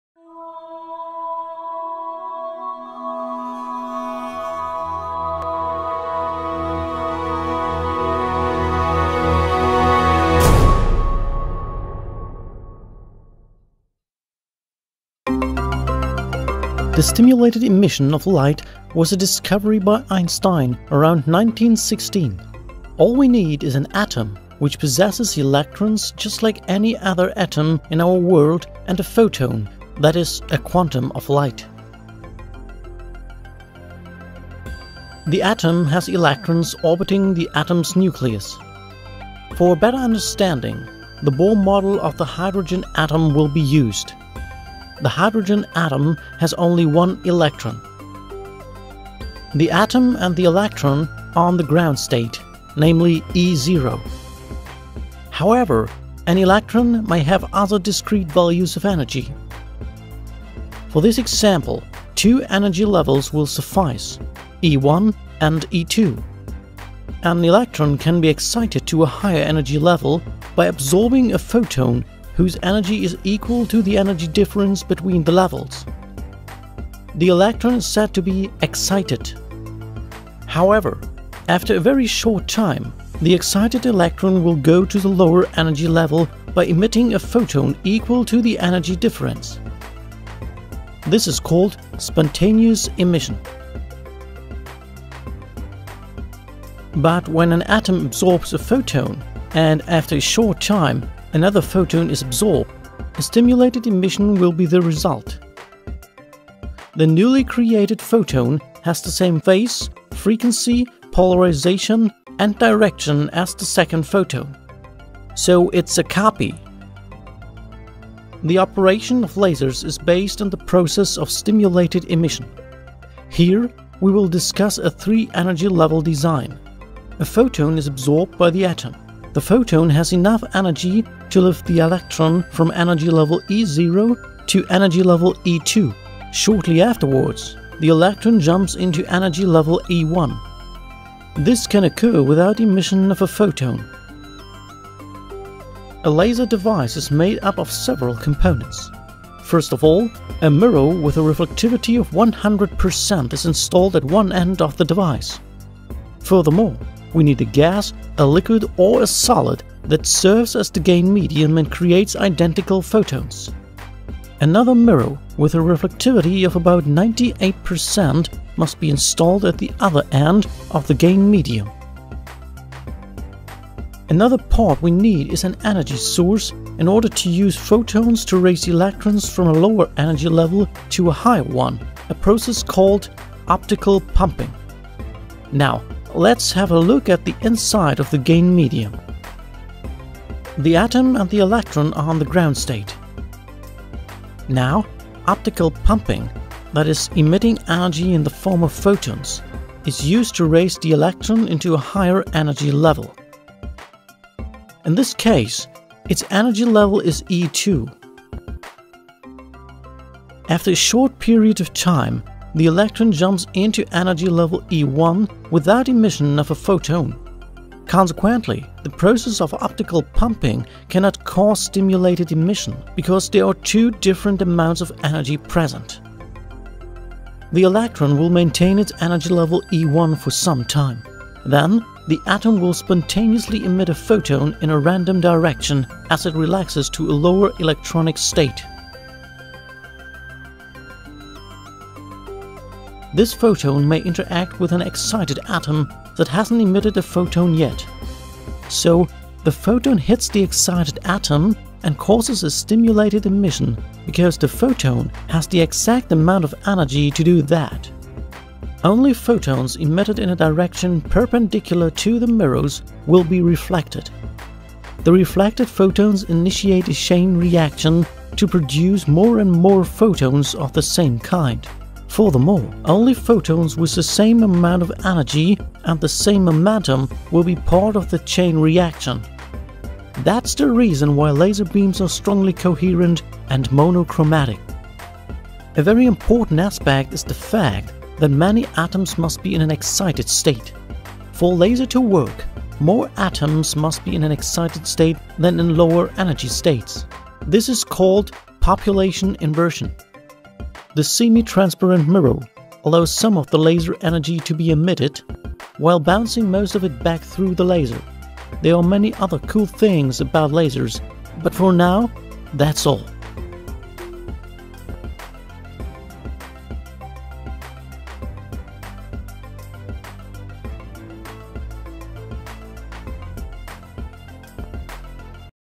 How LASERs work Animation with Einstein كيف يعمل الليزر
الليزر جهاز له استخدامات فى شتى المجالات بنيت فكرة عمله على نظرية الانبعاث المستحث للضوء المرئى قدم هذا النظرية الفيزيائى الشهير ألبرت اينشتين فى عام 1917 وظل تطبيق النظرية معلقا حتى العام 1960 اى بعد وفاة اينشتين بحوالى 5 سنوات حينما نجح العالم الامريكى ميمان فى تقديم أول ليزر فى تاريخ البشر.